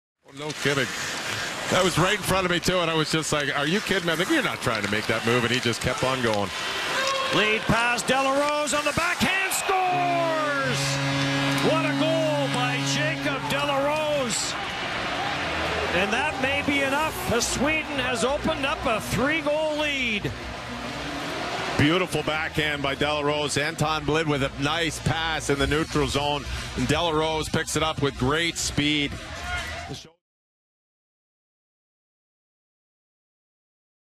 Jacob de la Rose Nice Goal vs Czech Republic (26/12/14)
Jacob de la Rose's (#9) (34th, 2nd, 2013) nice backhand goal versus the Czech Republic in the 2015 World Junior Championship.